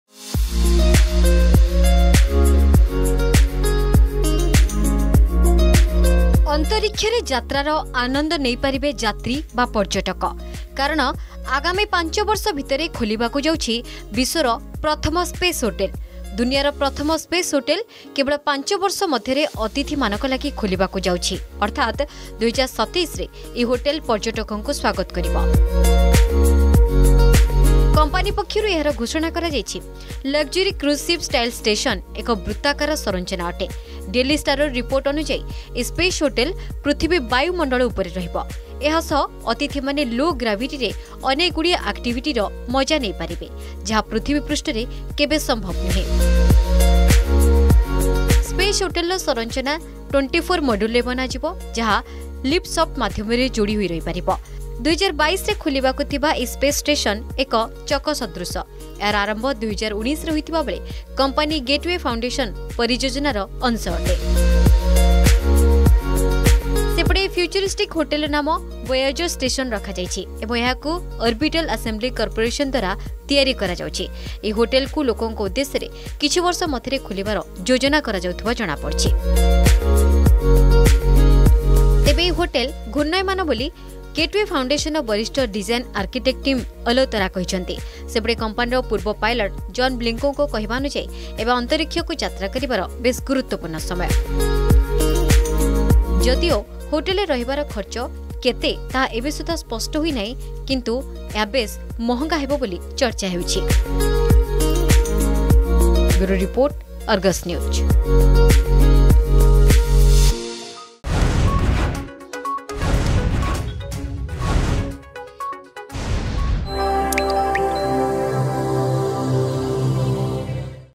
World’s first space hotel to open soon, common people will be able to enjoy the journey in space
The world’s first space hotel will open in five years i.e in 2027 and common people will be able to enjoy the journey in space.